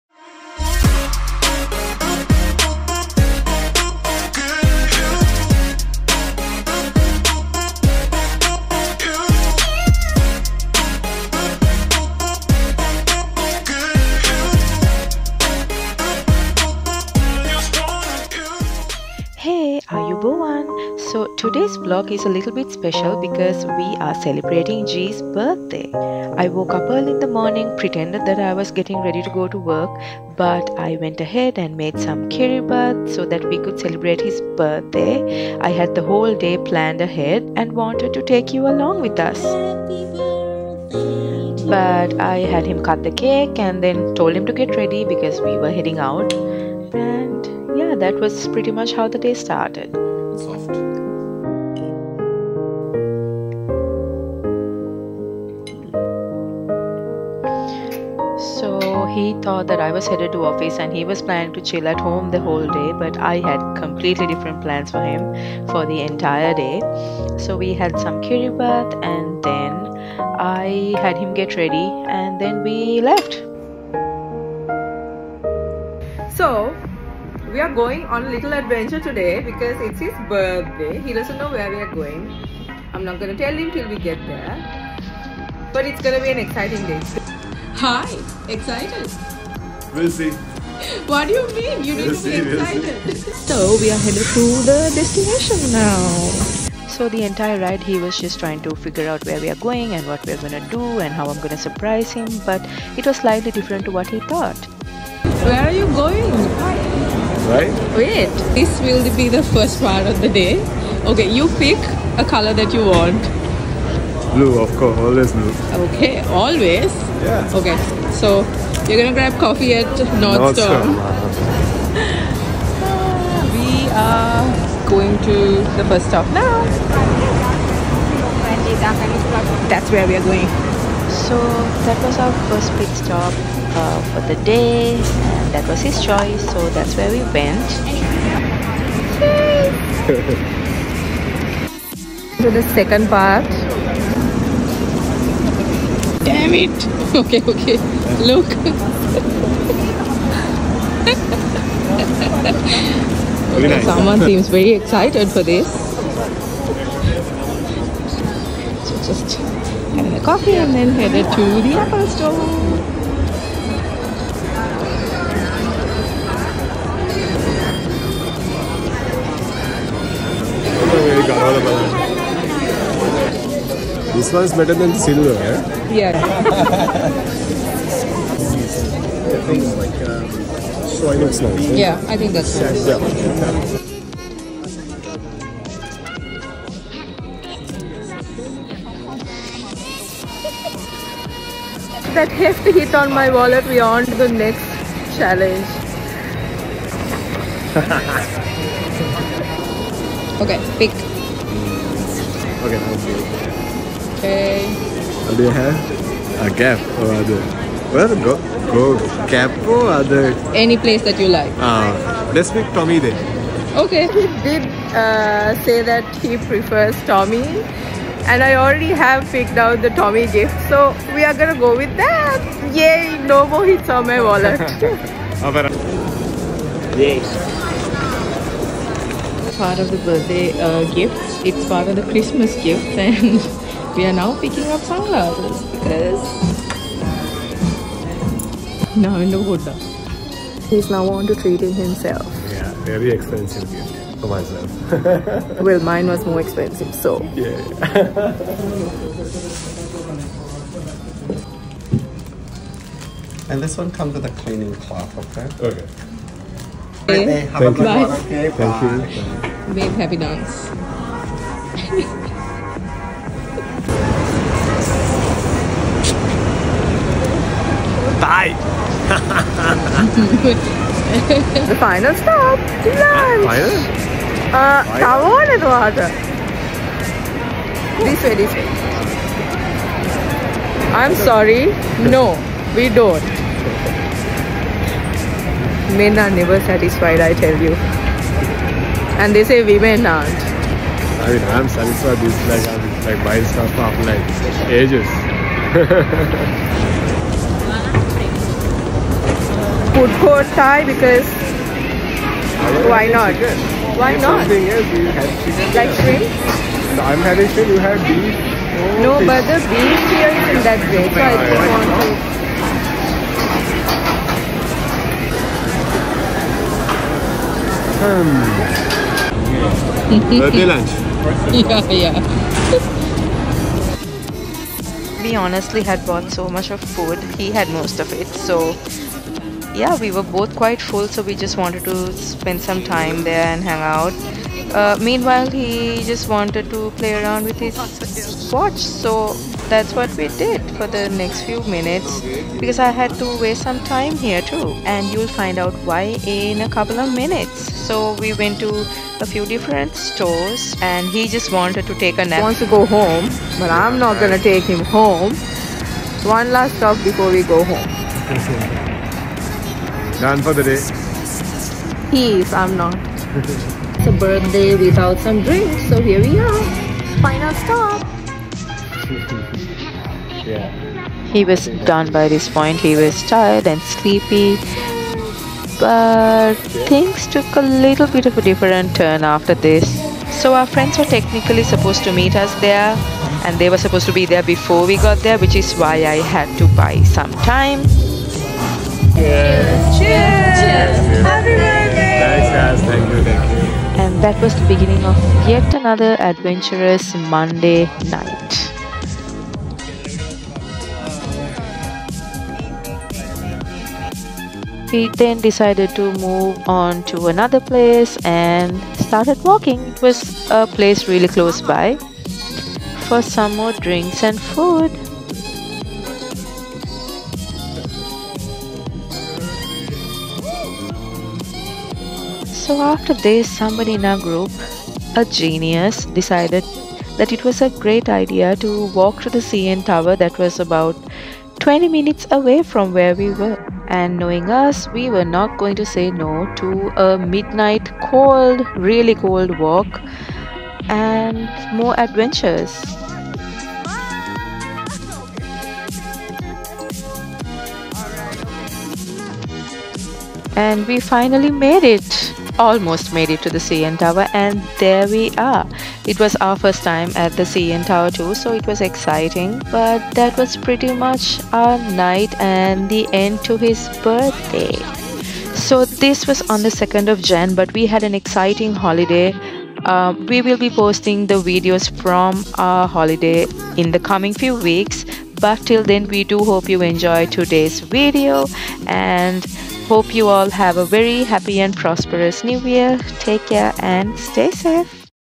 PICK A CARD - First Birthday In Canada
It was G's Birthday!!! Join us as we take you along this fun filled day.

His birthday is on 2nd of Jan and it was a holiday here in Canada, so we could spend the whole day together! G doesn't usually like celebrating his birthday and had plans of spendign the entire day at home and chilling, but hey, I do! :P

Which of his gifts were your favorites?

Also let us know if you prefer our videos in Portrait or Landscape mode.

Some more of our videos!

Wish you all a very happy and prosperous New year!

Xoxo
Mino & G